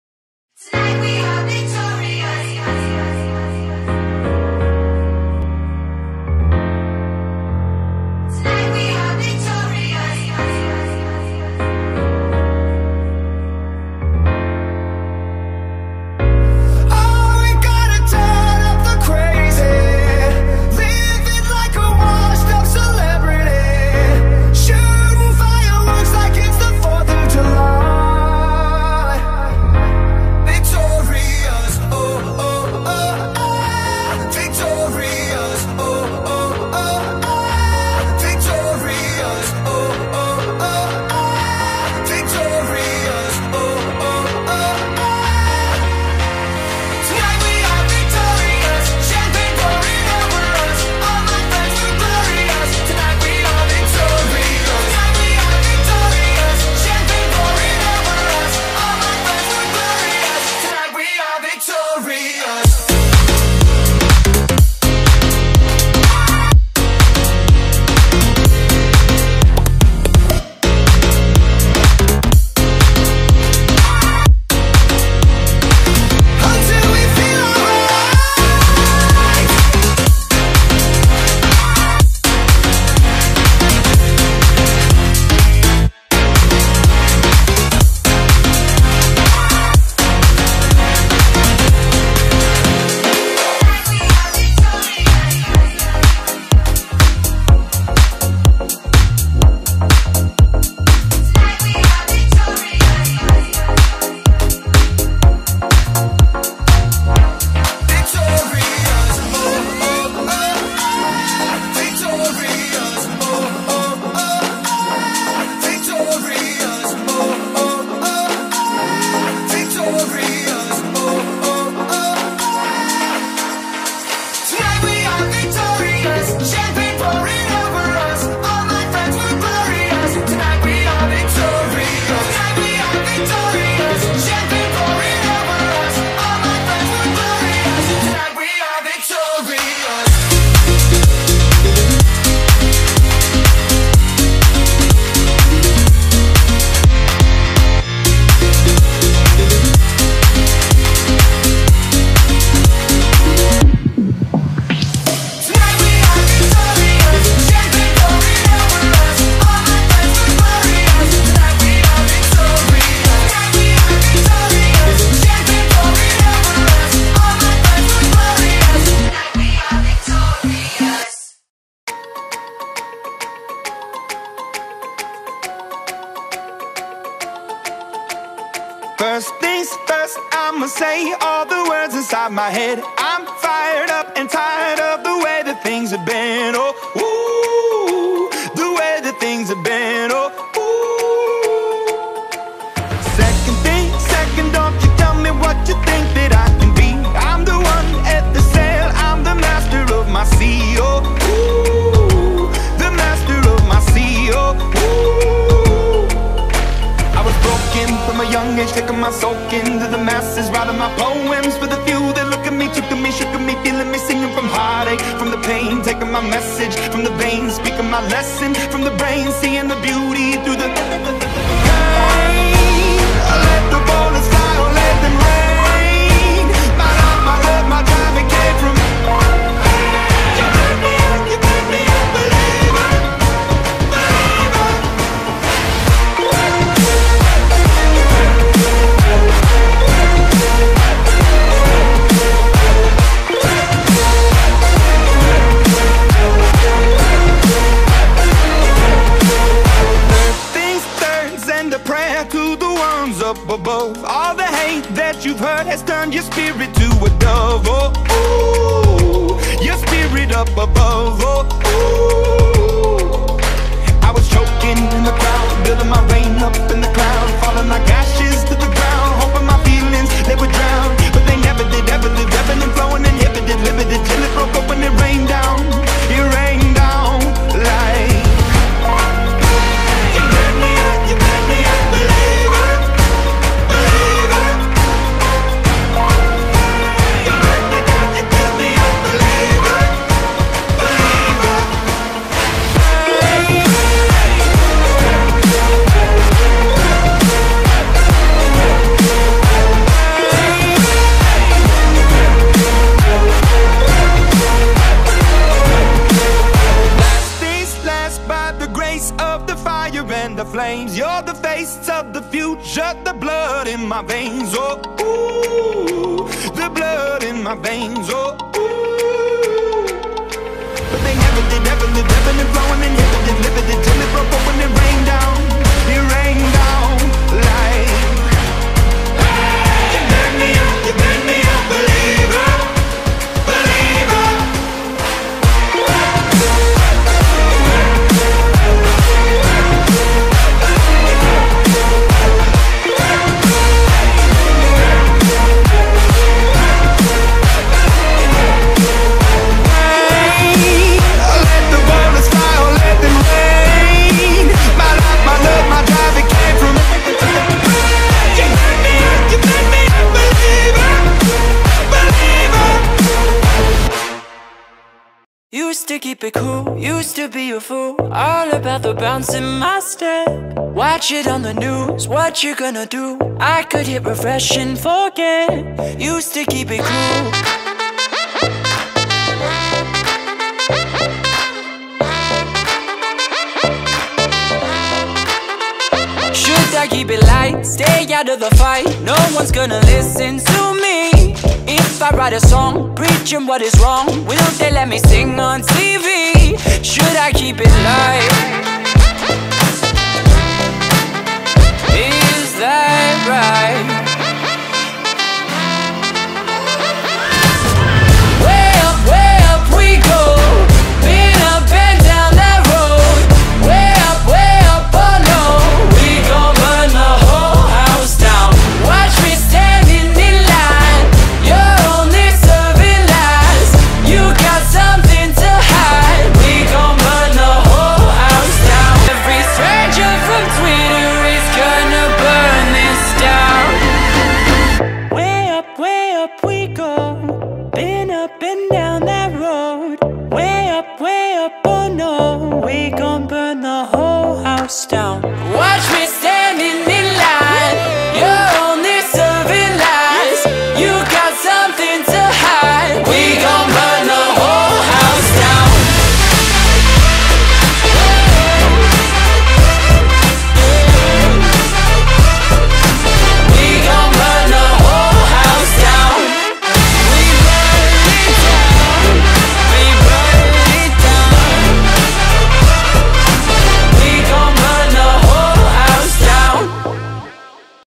2018 Weber State Soccer All Goals Scored
Every single goal scored by Weber State soccer in the 2018 season.

Follow the Wildcats on all the things.